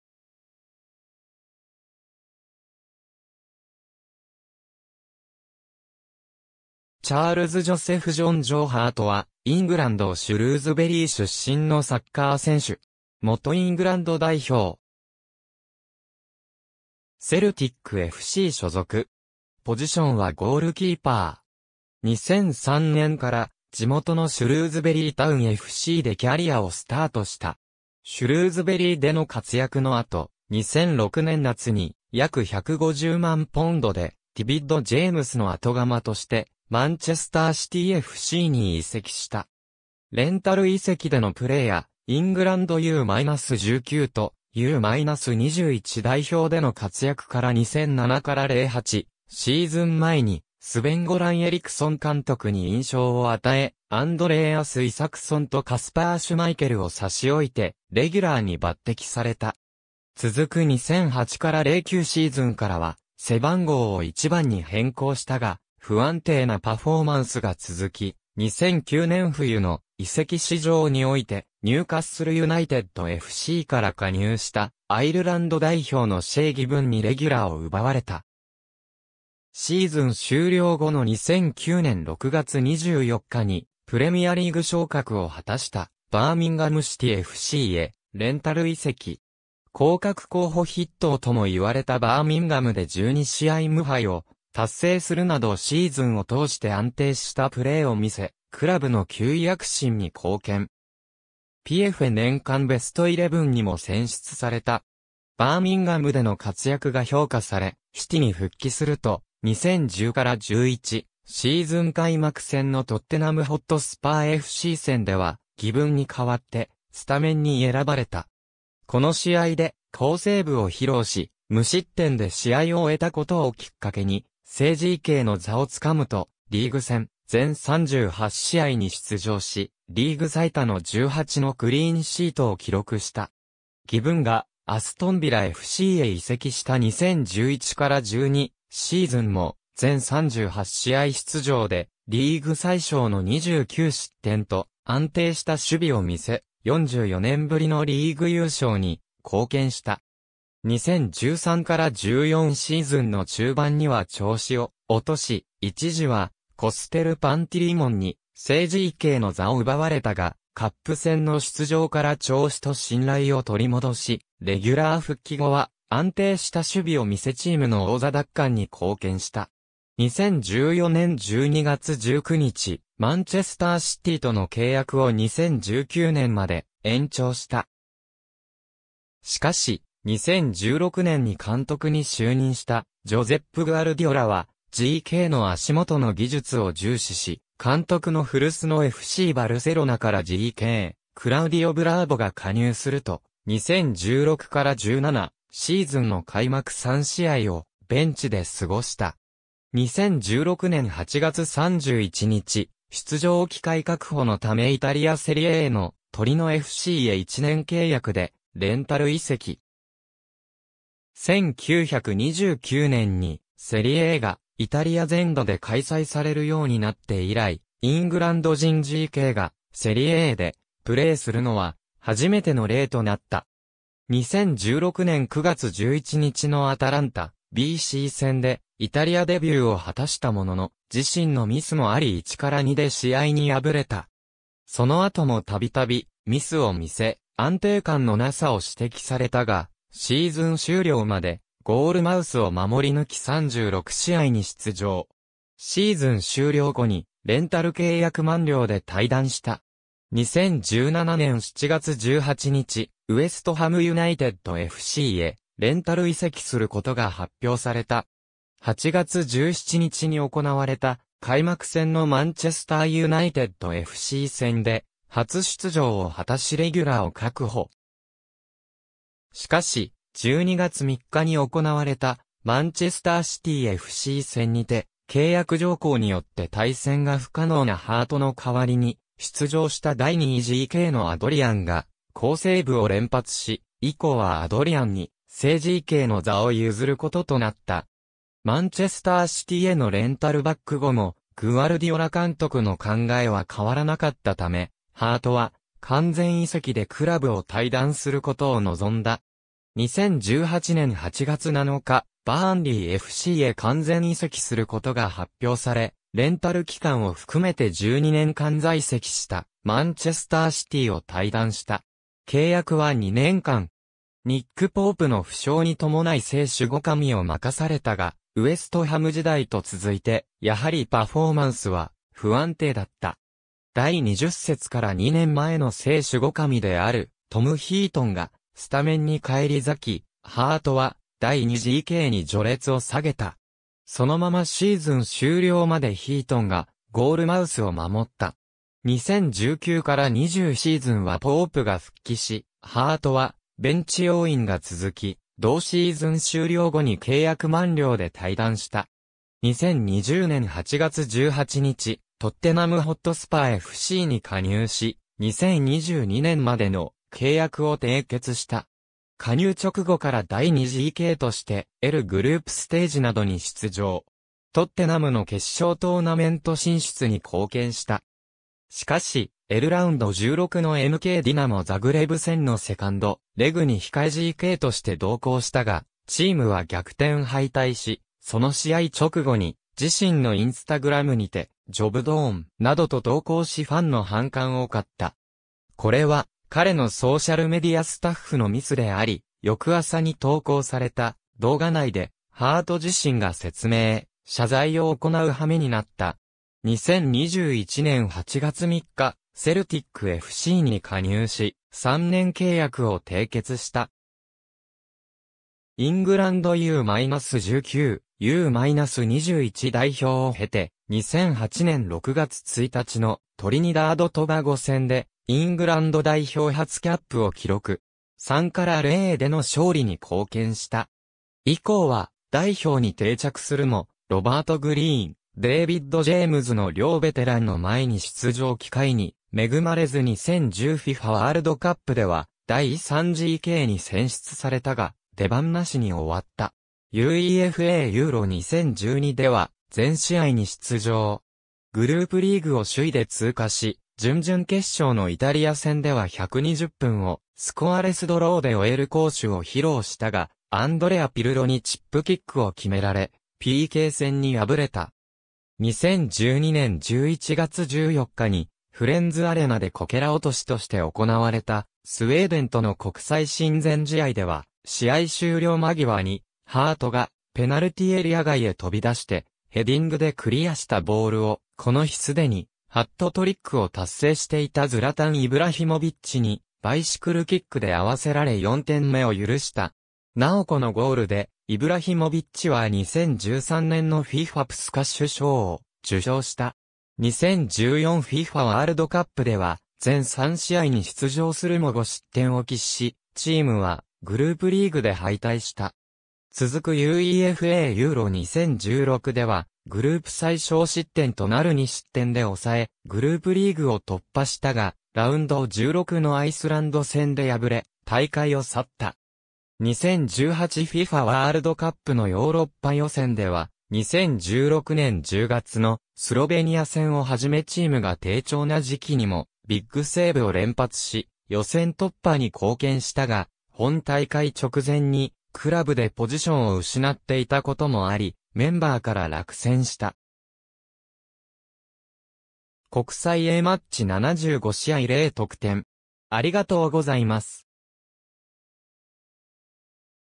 ジョー・ハート
シュルーズベリー・タウンFCの選手
チャールズ・ジョセフ・ジョン・"ジョー"・ハート（Charles Joseph John "Joe" Hart、1987年4月19日 - ）は、イングランド・シュルーズベリー出身のサッカー選手。
元イングランド代表。
セルティックFC所属。
2003年から地元のシュルーズベリー・タウンFCでキャリアをスタートした。
シュルーズベリーでの活躍の後、2006年夏に約150万ポンドでディビッド・ジェームスの後釜としてマンチェスター・シティFCに移籍した。
レンタル移籍でのプレーやイングランドU-19とU-21代表での活躍から2007-08シーズン前にスヴェン・ゴラン・エリクソン監督に印象を与え、アンドレーアス・イサクソンとカスパー・シュマイケルを差し置いてレギュラーに抜擢された。
続く2008-09シーズンからは背番号を1番に変更したが、不安定なパフォーマンスが続き、2009年冬の移籍市場においてニューカッスル・ユナイテッドFCから加入したアイルランド代表のシェイ・ギヴンにレギュラーを奪われた。
シーズン終了後の2009年6月24日に、プレミアリーグ昇格を果たしたバーミンガム・シティFCへレンタル移籍。
降格候補筆頭とも言われたバーミンガムで12試合無敗を達成するなどシーズンを通して安定したプレーをみせ、クラブの9位躍進に貢献。
PFA年間ベストイレブンにも選出された。
バーミンガムでの活躍が評価されシティに復帰すると、2010-11シーズン開幕戦のトッテナム・ホットスパーFC戦では、ギヴンに代わってスタメンに選ばれた。
この試合で好セーブを披露し無失点で試合を終えたことをきっかけに正GKの座を掴むと、リーグ戦全38試合に出場し、リーグ最多の18のクリーンシートを記録した。
ギヴンがアストン・ヴィラFCへ移籍した2011-12シーズンも全38試合出場でリーグ最少の29失点と安定した守備をみせ、44年ぶりのリーグ優勝に貢献した。
2013-14シーズンの中盤には調子を落とし、一時はコステル・パンティリモンに正GKの座を奪われたが、カップ戦の出場から調子と信頼を取り戻し、レギュラー復帰後は安定した守備を見せチームの王座奪還に貢献した。
2014年12月19日、マンチェスター・シティとの契約を2019年まで延長した。
しかし、2016年に監督に就任したジョゼップ・グアルディオラはGKの足元の技術を重視し、監督の古巣のFCバルセロナからGKクラウディオ・ブラーボが加入すると、2016-17シーズンの開幕3試合をベンチで過ごした。
2016年8月31日、出場機会確保のためイタリア・セリエAのトリノFCへ1年契約でレンタル移籍。
1929年にセリエAがイタリア全土で開催されるようになって以来、イングランド人GKがセリエAでプレーするのは初めての例となった。
2016年9月11日のアタランタBC戦でイタリアデビューを果たしたものの、自身のミスもあり1-2で試合に敗れた。
その後もたびたびミスを見せ、安定感のなさを指摘されたが、シーズン終了までゴールマウスを守り抜き36試合に出場。
シーズン終了後にレンタル契約満了で退団した。
2017年7月18日、ウェストハム・ユナイテッドFCへレンタル移籍することが発表された。
8月17日に行われた開幕戦のマンチェスター・ユナイテッドFC戦で初出場を果たしレギュラーを確保。
しかし、12月3日に行われたマンチェスター・シティFC戦にて、契約条項によって対戦が不可能なハートの代わりに出場した第2GKのアドリアンが好セーブを連発し、以降はアドリアンに正GKの座を譲ることとなった。
マンチェスター・シティへのレンタルバック後もグアルディオラ監督の考えは変わらなかったため、ハートは完全移籍でクラブを退団することを望んだ。
2018年8月7日、バーンリーFCへ完全移籍することが発表され、レンタル期間を含めて12年間在籍したマンチェスター・シティを退団した。
契約は2年間。
ニック・ポープの負傷に伴い正守護神を任されたが、ウェストハム時代と続いてやはりパフォーマンスは不安定だった。
第20節から2年前の正守護神であるト...